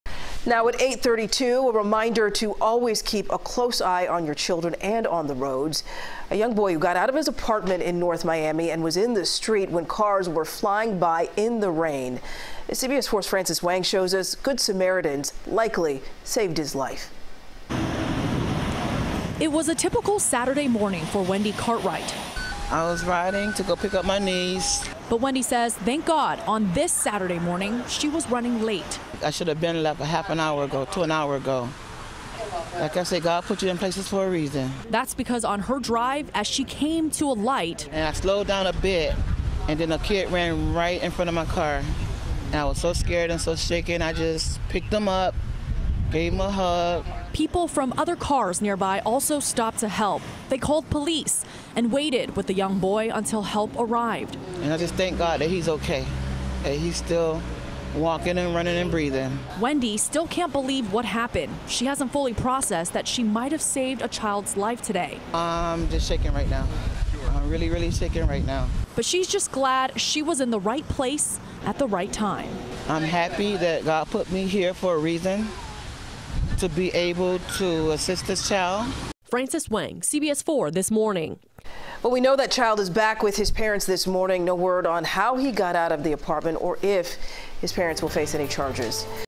Good Samaritans Rescue Child Who Wandered Off From North Miami Apartment
CBS4's Frances Wang spoke to the woman who spotted the boy.